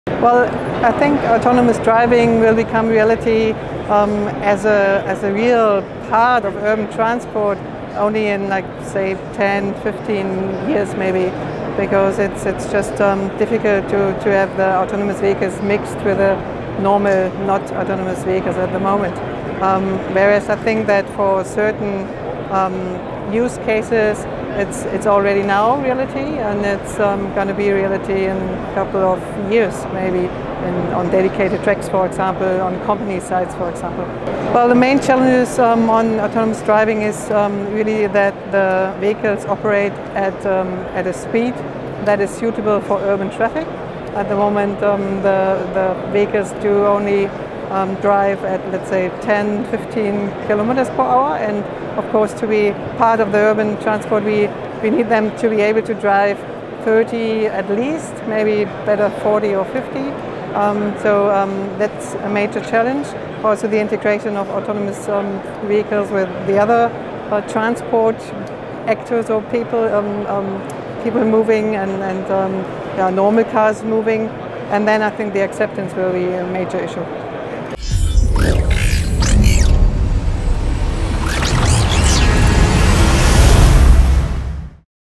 Tina Wagner About the Major Challenges for Autonomous Driving
Even though autonomous driving is already a reality on dedicated lanes or closed areas, according to Tina Wagner, transport expert of the city of Hamburg, there is still some way to go before driverless vehicles are an integral part of urban transport. Watch the video and learn more about the underlying challenges that need to be mastered.